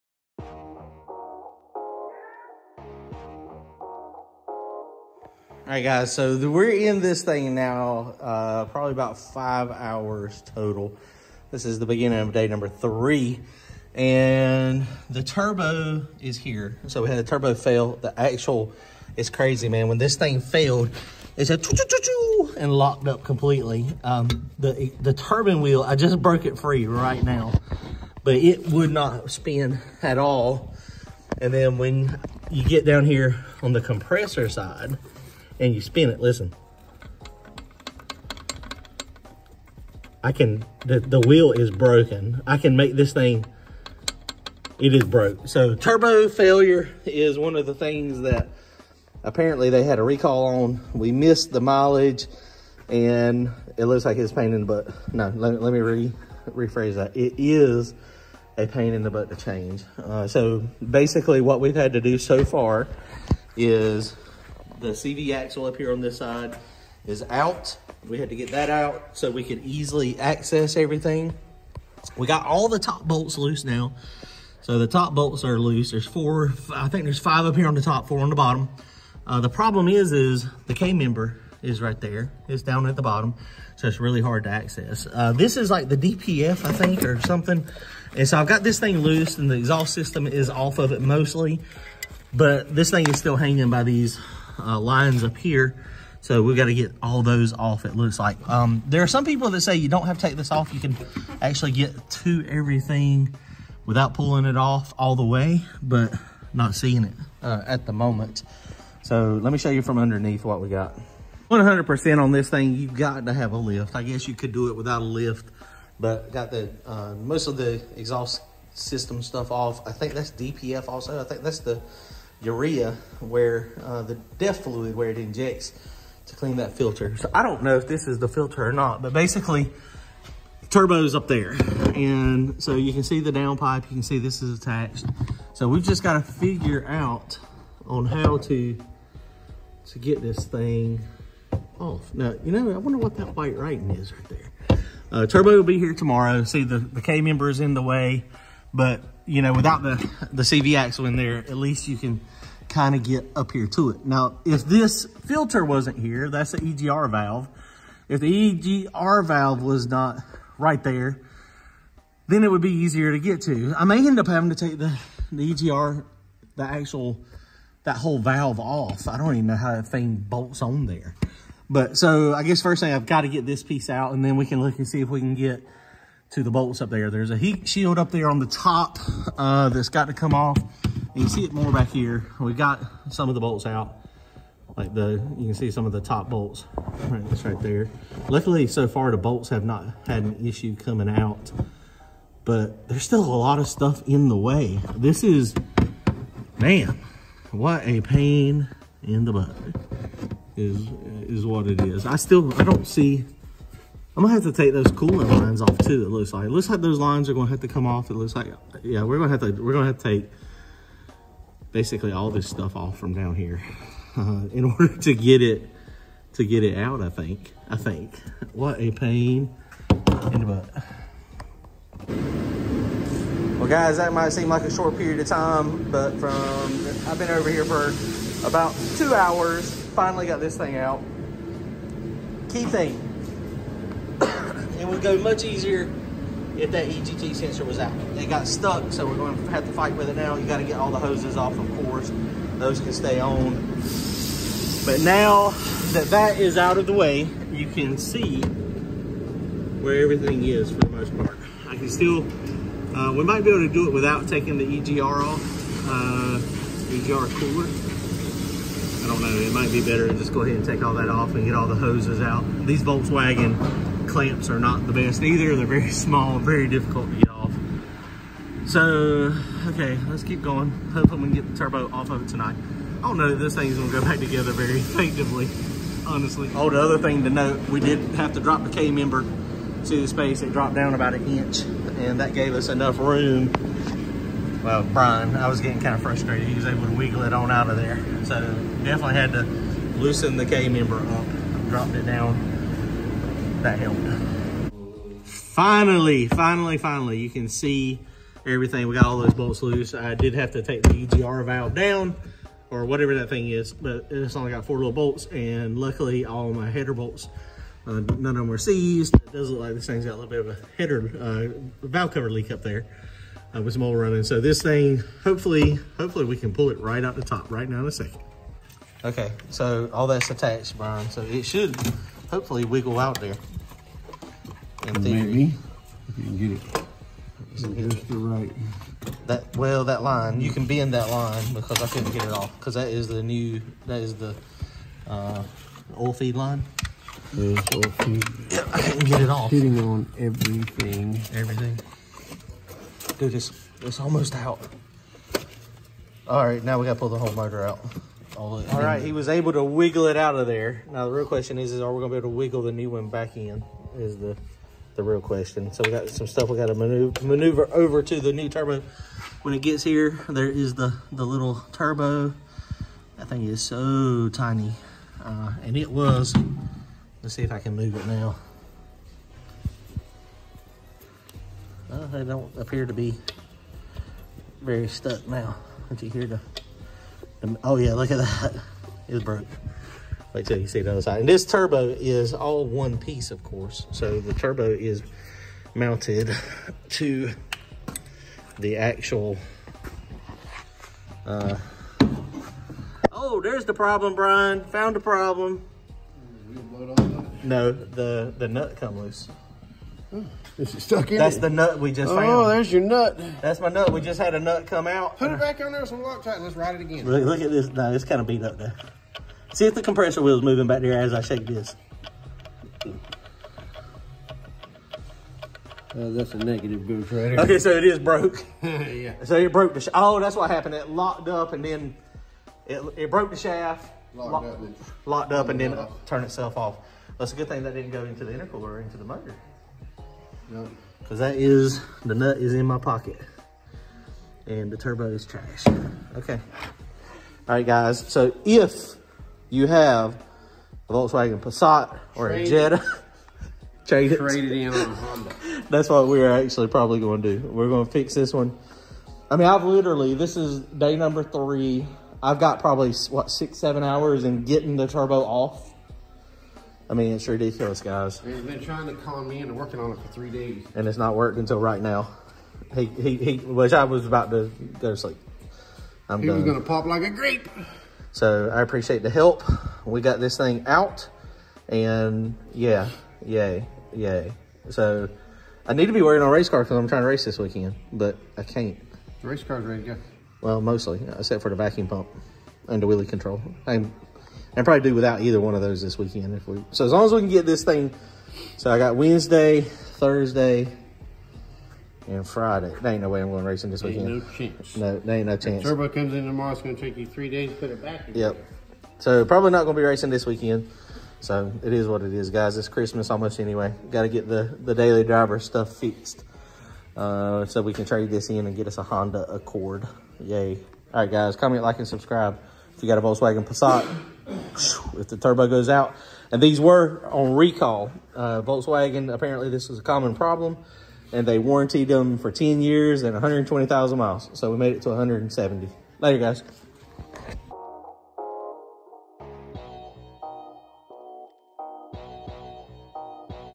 VW Passat TDI 2.0 Turbo Failure
There was a recall on this particular turbo, on my 2012 Volkswagen Passat TDI.  The extended warranty was for 10 years, or 120,000 miles.  This one lasted 173,000 before it failed.  This was my first attempt at replacing.  The removal process was very time consuming and not an easy process at all.  Hopefully the install goes s little smoother and quicker!